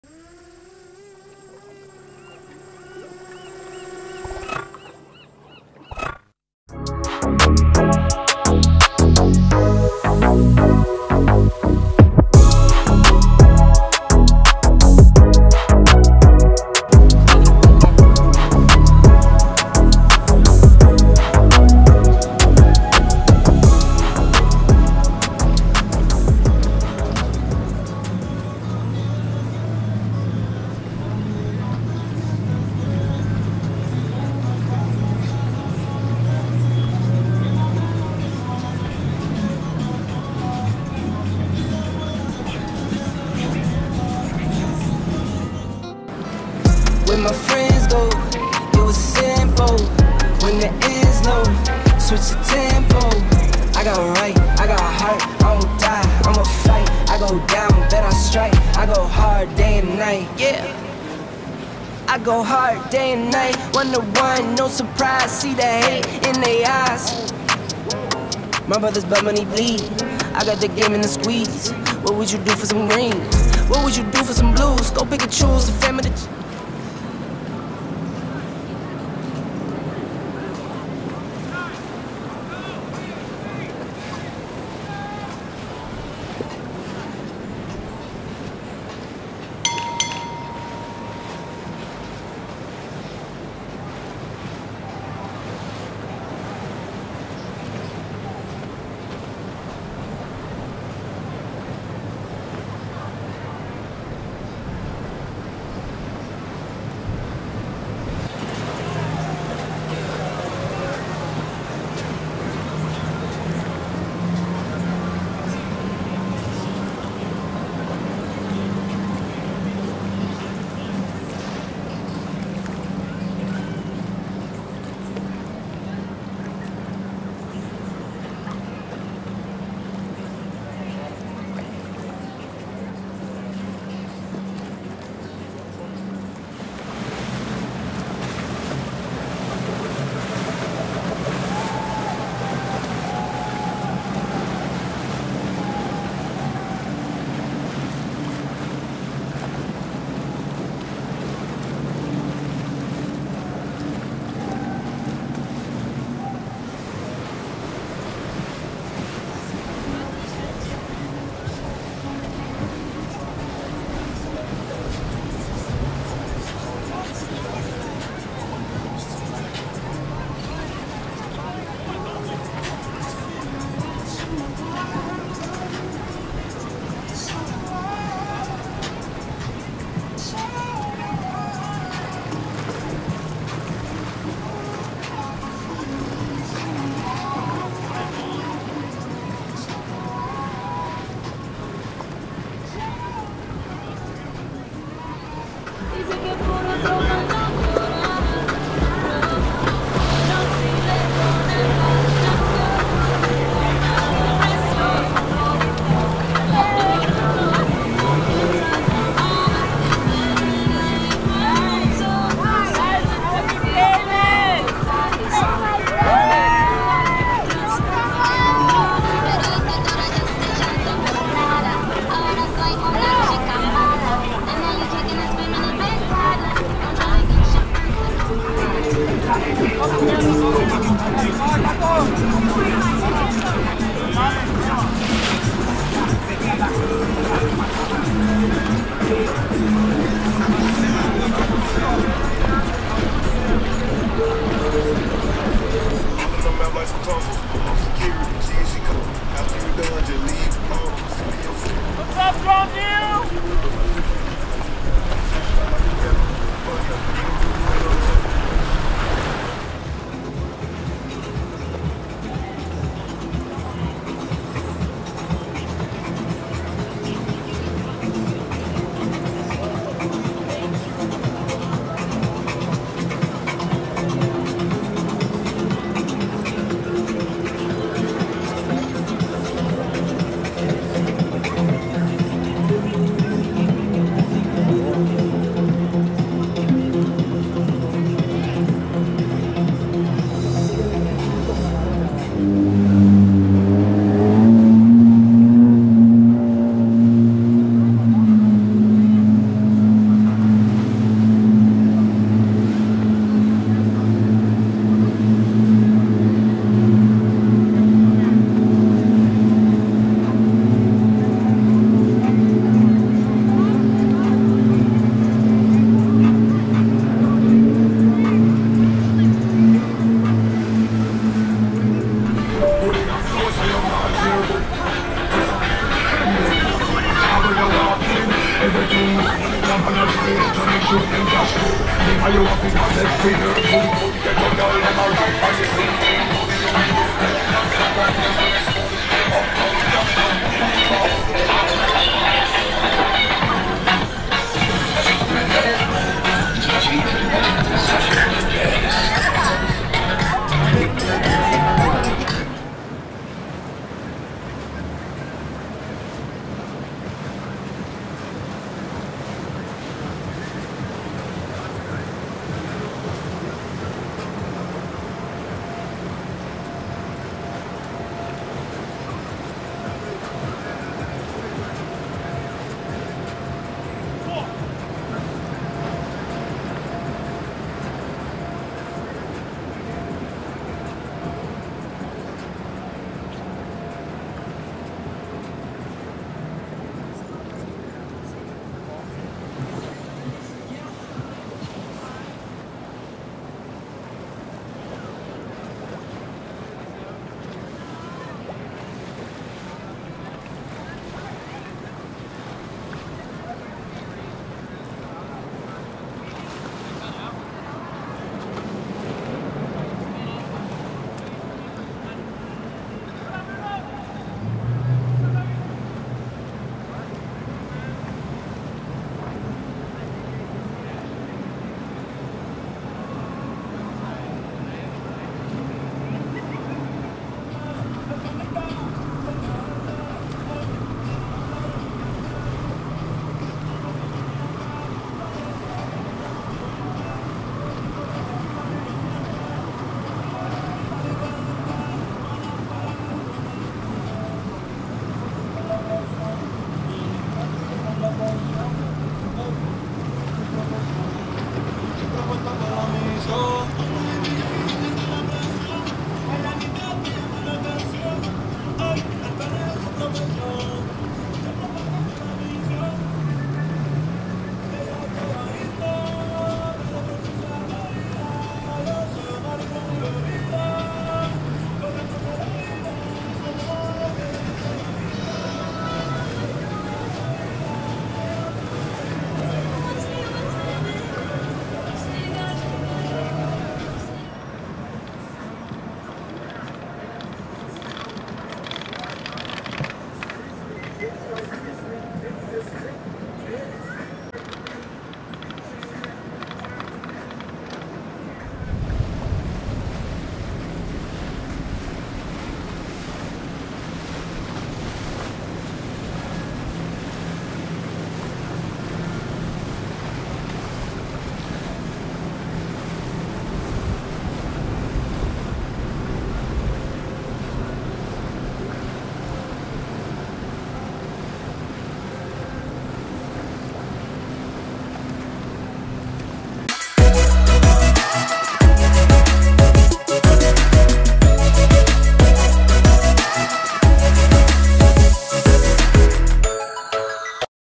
Best Gunnel Rider by Far!! A little Unsafe by the way | Miami River |Droneviewhd [ Boats & Yachts ]
Another great day at the Miami River full of beautiful yachts, crazy and fun people.. We spotted a few fans, Thank you guys for the support and we also caught a few interesting people doing what they do best... having fun on the water.